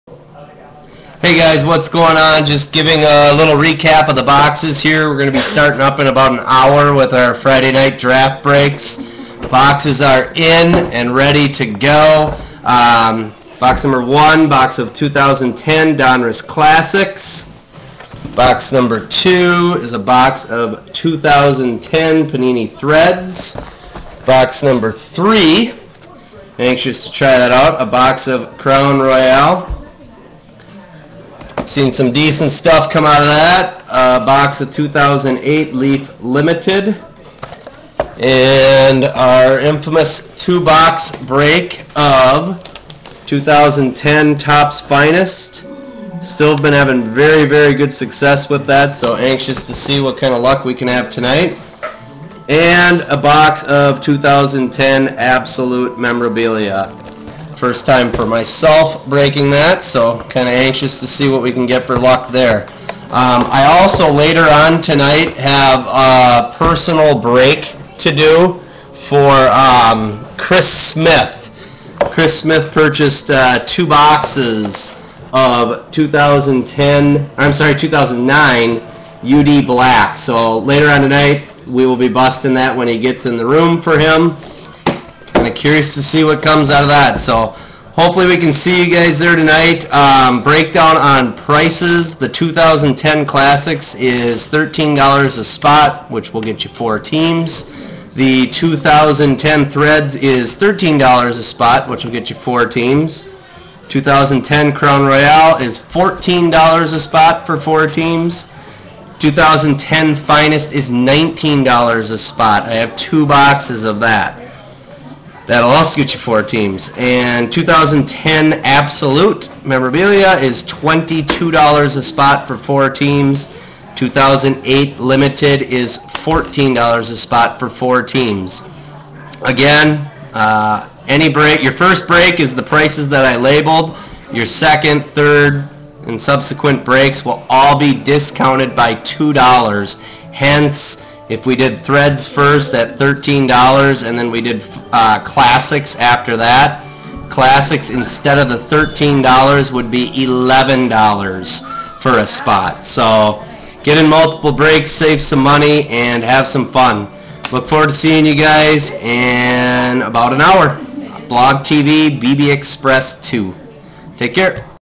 Football Draft Box Break Tonight BlogTV 8pm CST BBEXPRESS2
Friday night football draft box breaks.  I will have the following boxes:
2010 Threads, Classics, Crown Royale, Absolute, 2 boxes of Finest, and 2008 Limited